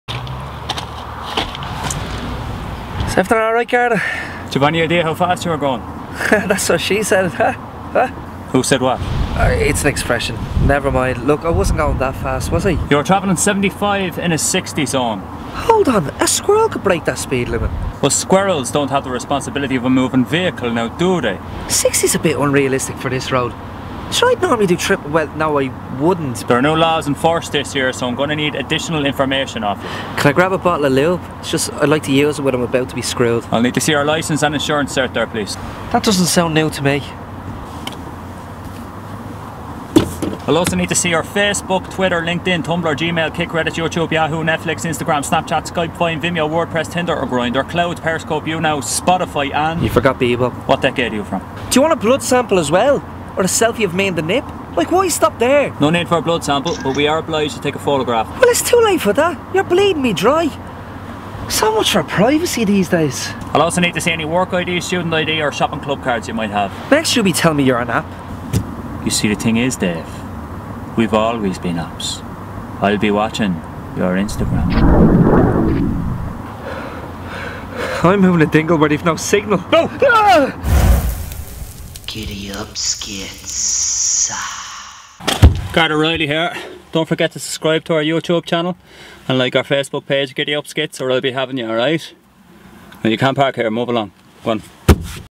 Social Media Checkpoint
Garda pulls over a car and asks the driver for his Social Media details. But is he real or just an App?